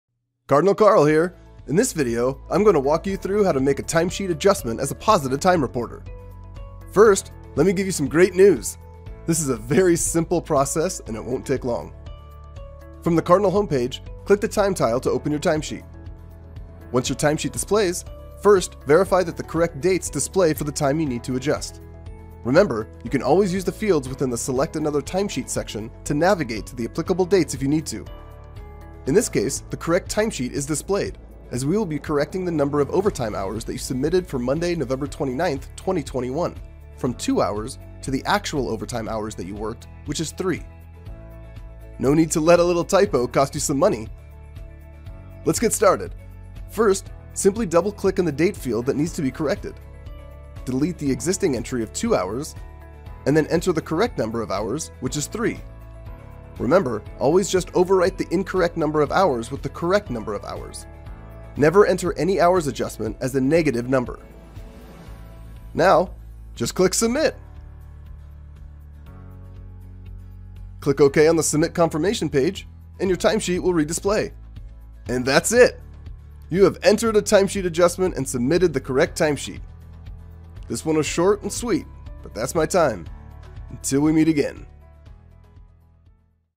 Entering a Timesheet Adjustment (Positive Time Reporter)
Positive Time Reporters who have modify access to their Timesheet using Cardinal can use these step-by-step instructions on how to enter and submit a Timesheet adjustment when corrections to the Timesheet are required.